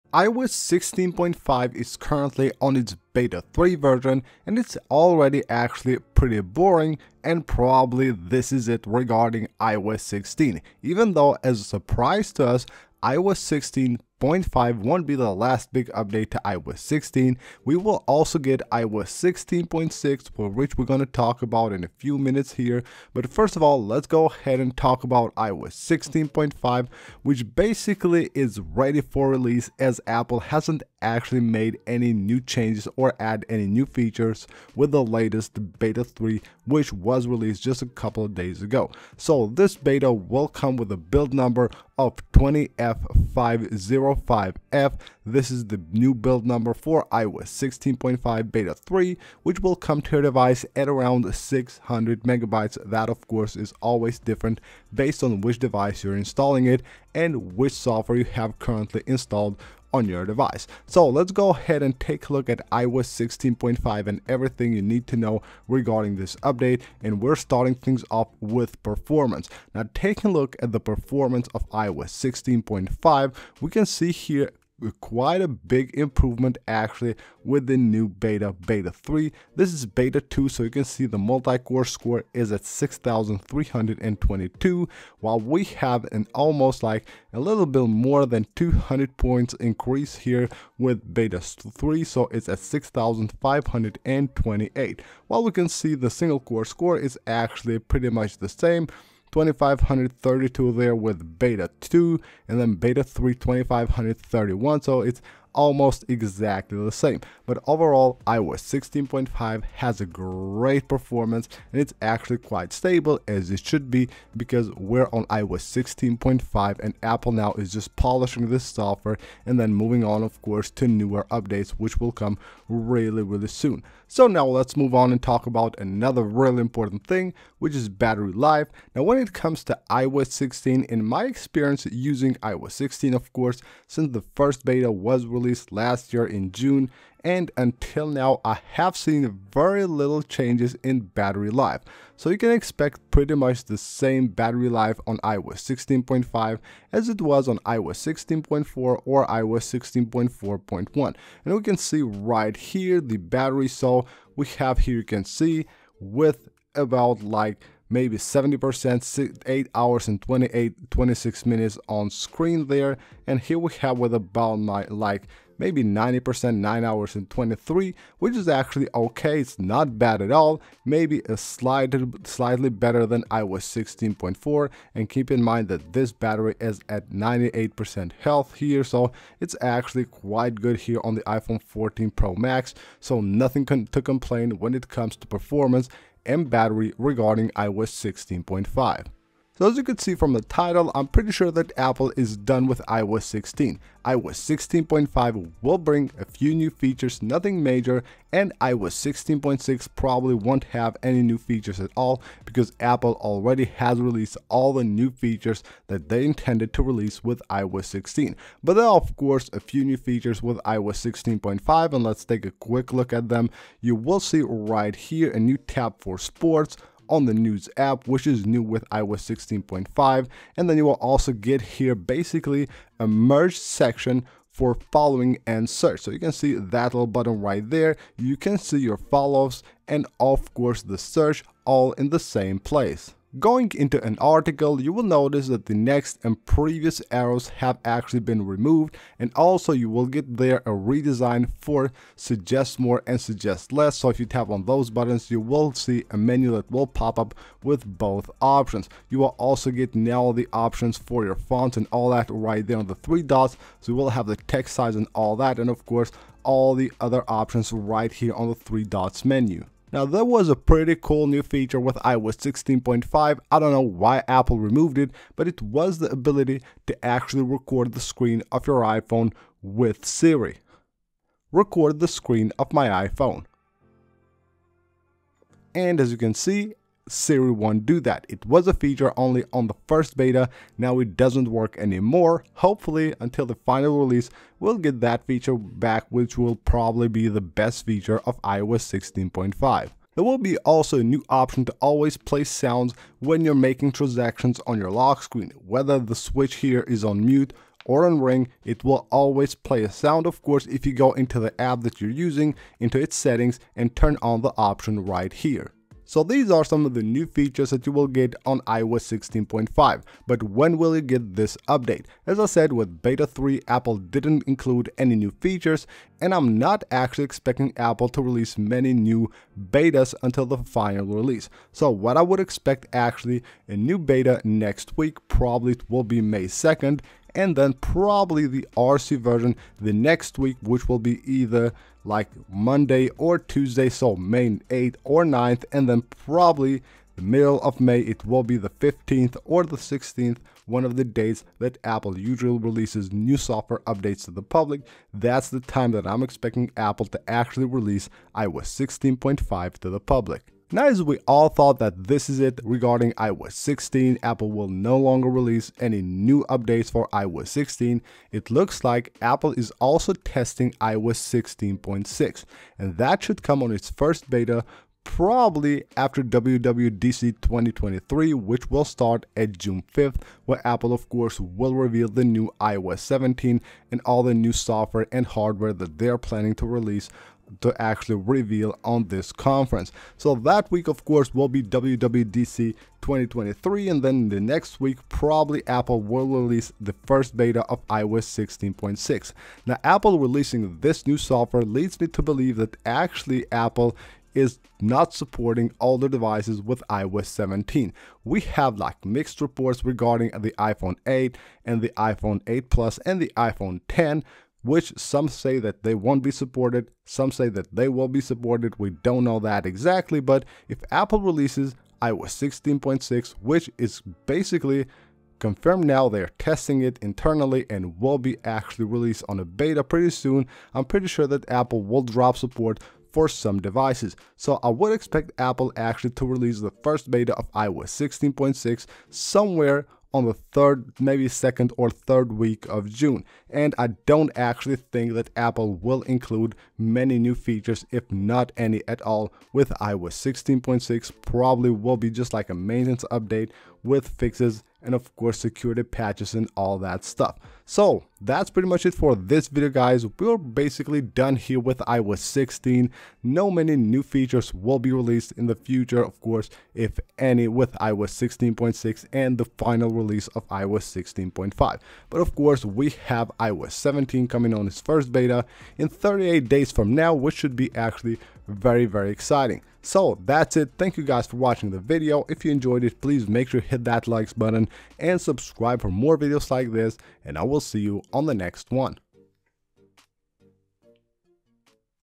iOS 16.5 & iOS 16.6 - We're ALL SET !
iOS 16.5 and iOS 16.6 will be the last updates to iOS 16. iOS 16.5 bring a few new features to iOS 16 and there will be another update which is iOS 16.6

In this video you can learn everything you need to know about the last updates to iOS 16 which should be released very soon and will mark the end of iOS 16.

New beta 0:00
Performance 1:01
Battery 2:04
Features 3:09
Next beta & Release 5:06
iOS 16.6 5:50